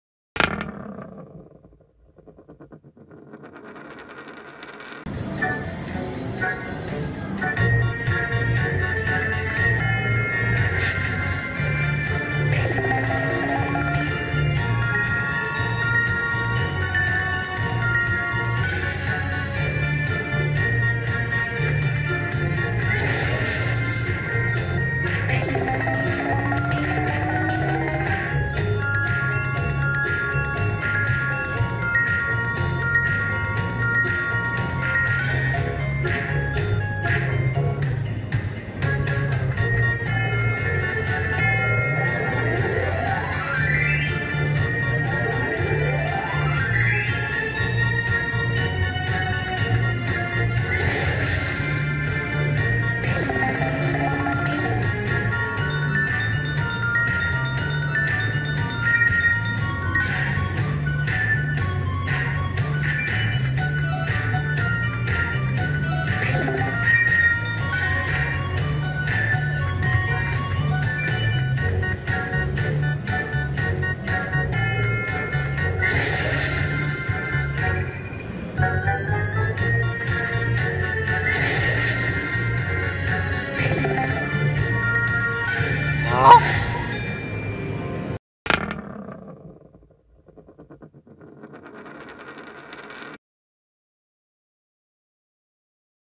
Lil Red slot machine bonus fail
The average bonus round for L'il Red is between 10x and 20x.  This bonus falls into this range, but I consider it a fail.
"Random $$ Slots" aka "Random $$$"